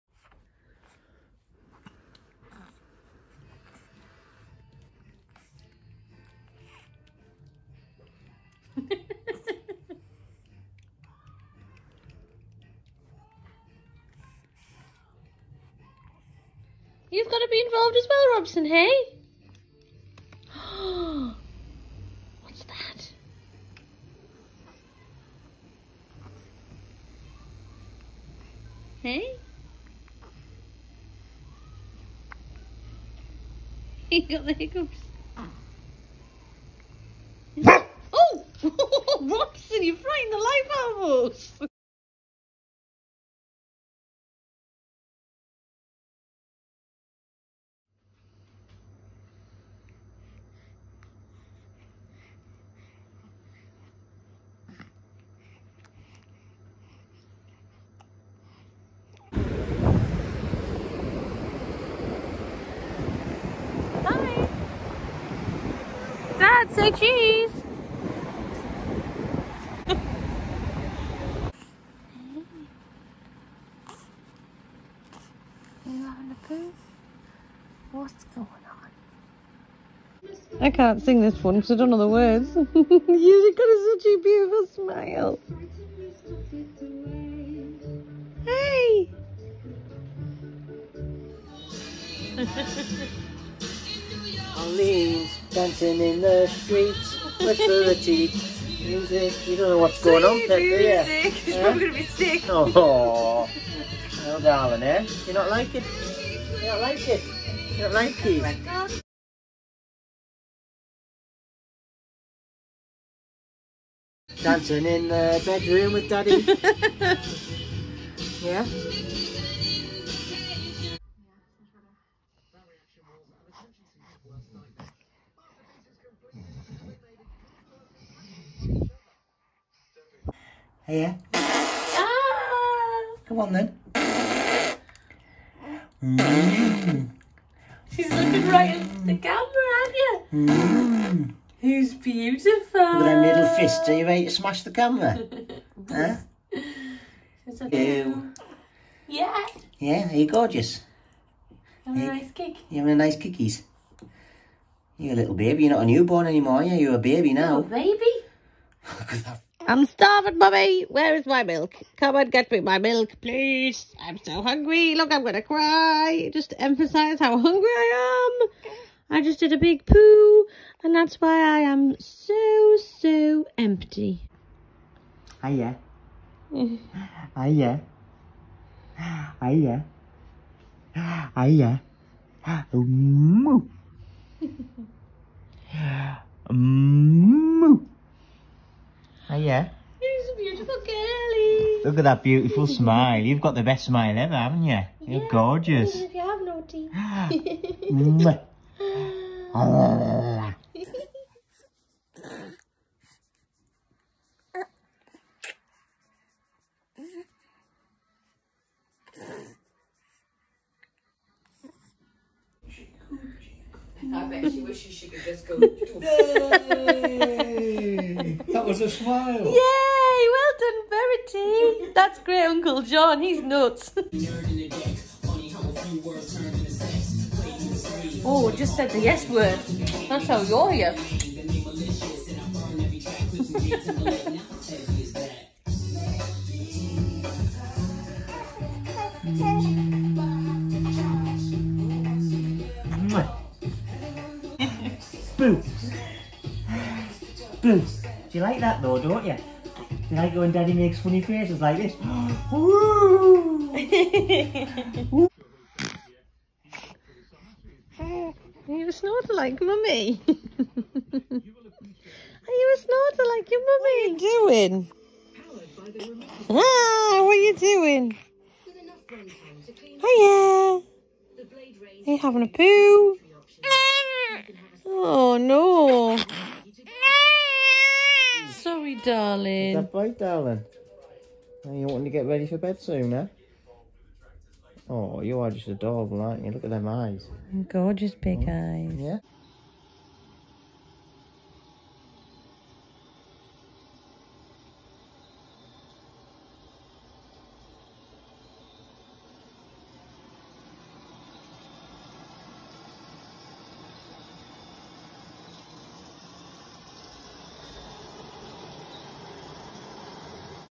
Verity - week 7
A week in the life of baby Verity.